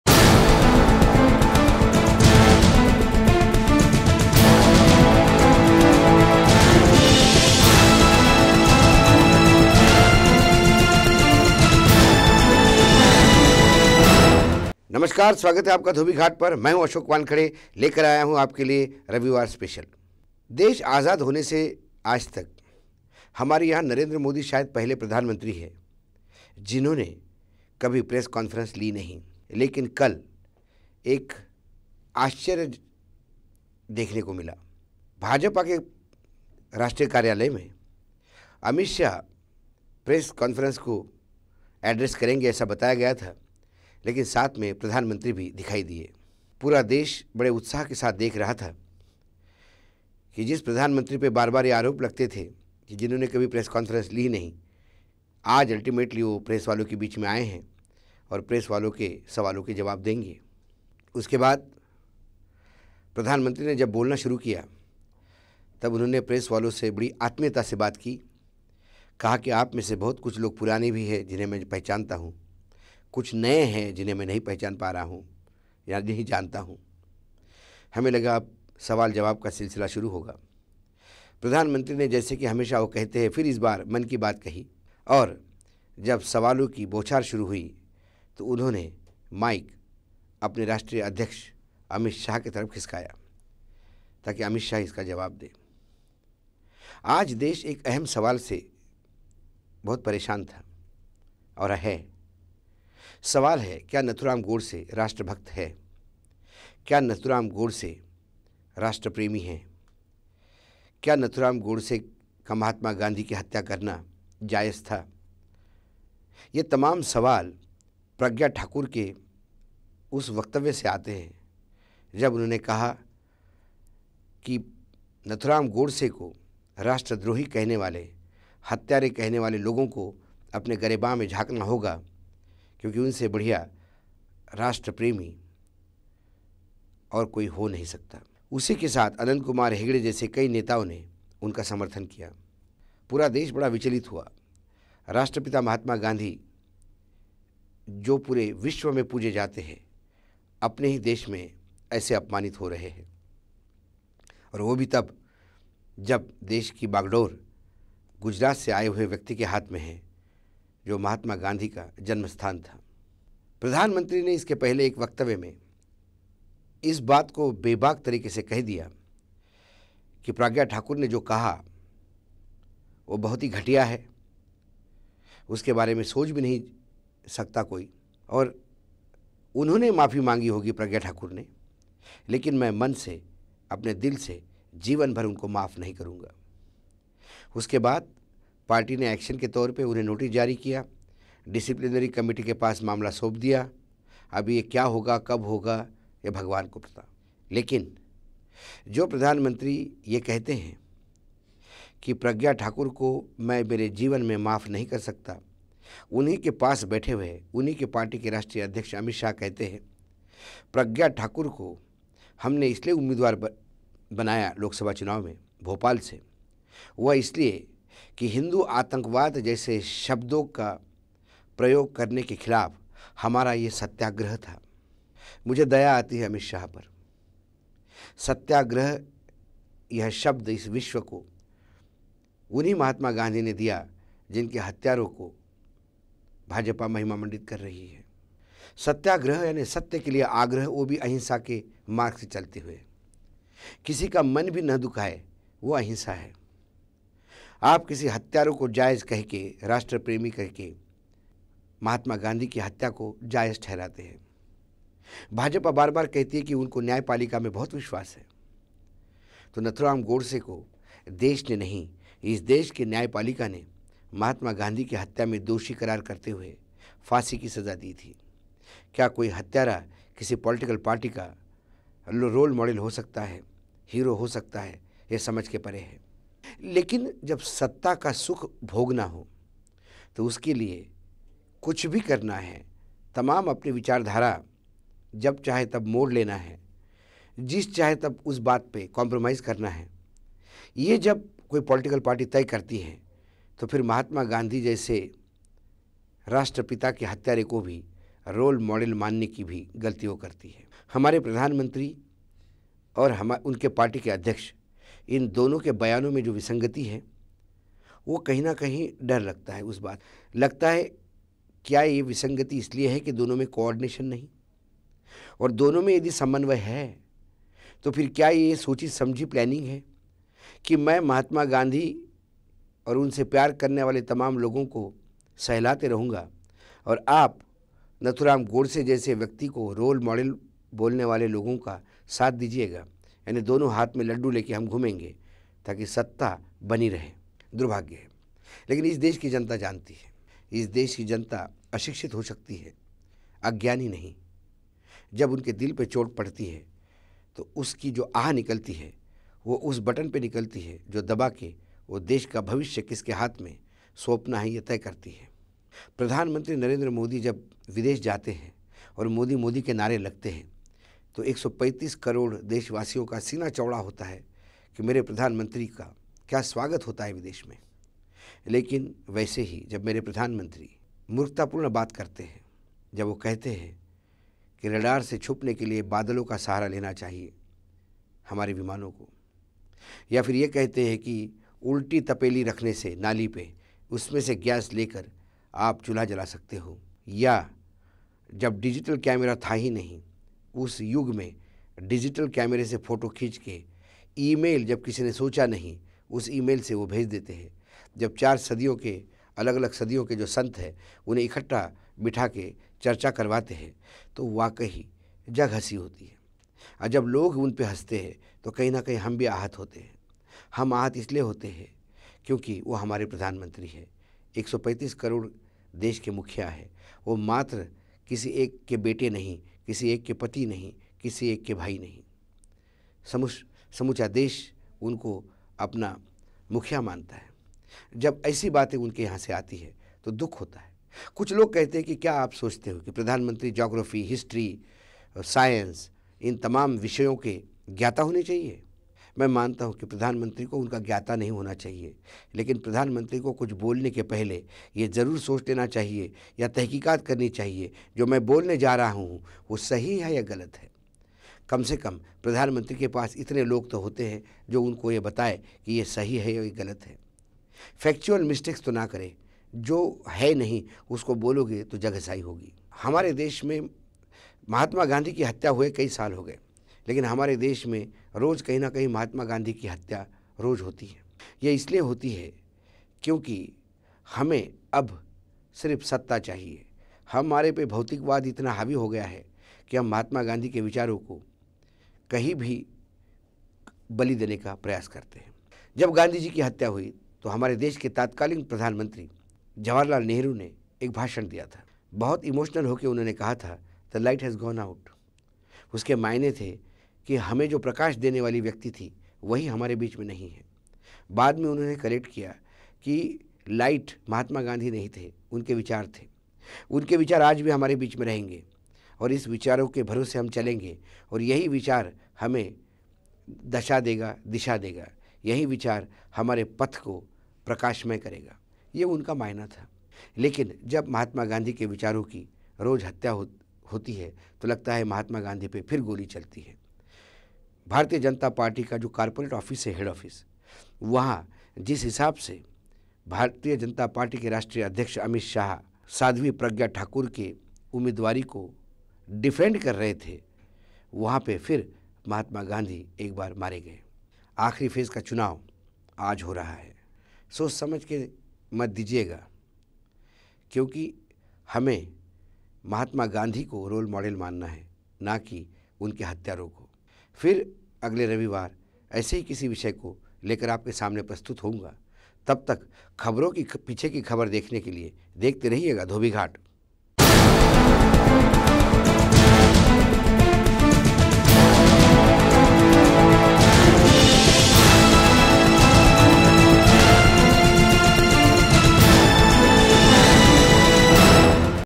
गोडसे, गांधी और प्रज्ञा
भाजपा की लोकसभा उम्मीदवार ठाकुर ने फिर से कहा नाथूराम गोडसे जिन्होंने महात्मा गांधी की हत्या की वह देश भक्त थे. मोदी ने कहा प्रज्ञा  को माफ नहीं करेंगे. अमित शाह ने कहा के खिलाफ प्रज्ञा की उम्मीदवारी यह हमारा सत्याग्रह था. एक बार प्रज्ञा ने महात्मा गांधी को मारा ,महात्मा गांधी की हत्या एक बार और भाजपा की राष्ट्रीय कार्यालय में हुई. क्या गोडसे राष्ट्रभक्त हो सकता है. इस पर ही रविवार विशेष. खबरों के पीछे की खबर देखने के लिए देखते रहिए धोबी घाट अशोक वानखड़े के साथ.

महात्मा गांधी
# सत्याग्रह
# नाथूराम गोडसे